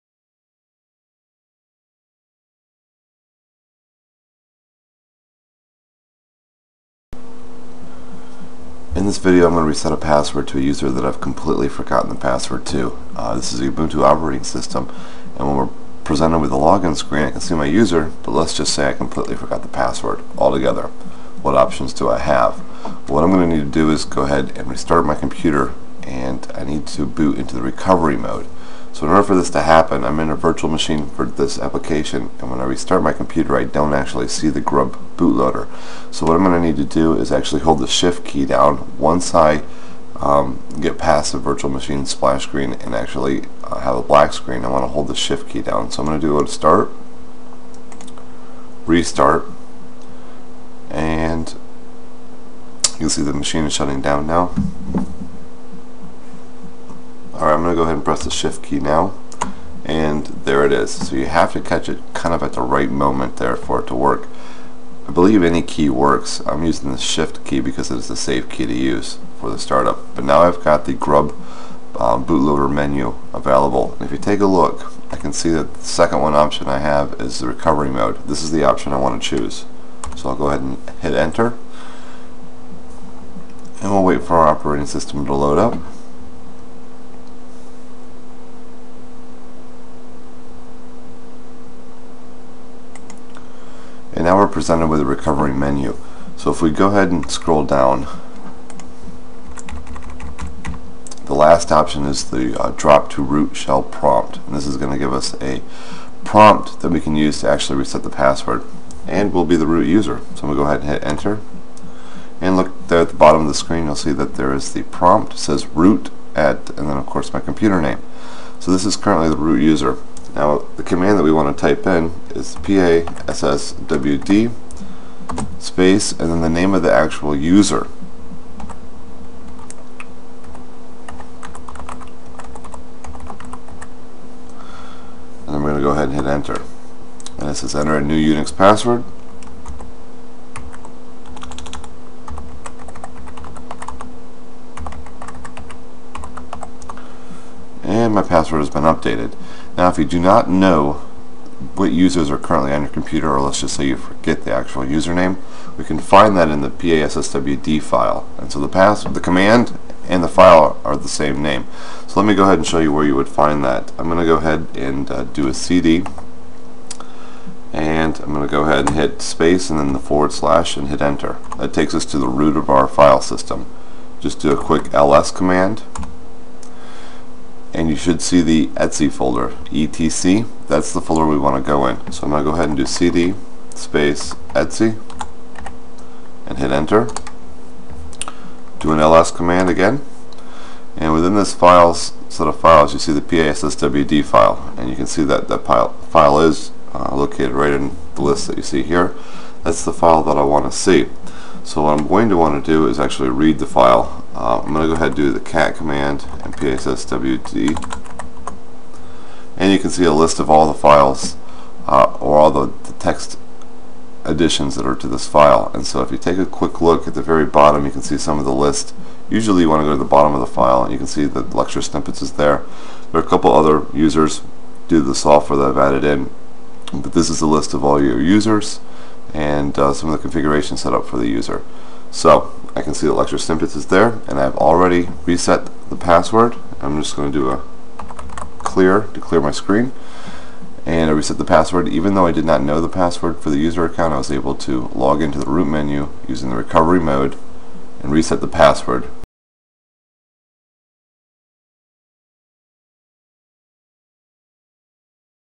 Resetting a Forgotten Password in Ubuntu
In this video I reset the password for an account where I have forgotten the password and can not log into the system in Ubuntu Linux.  I restart the computer and choose the recovery mode from the grub boot options. From there I choose the root user prompt and change the password using the passwd command.  I also load the /etc/passwd file using the cat command to view the user accounts for the system.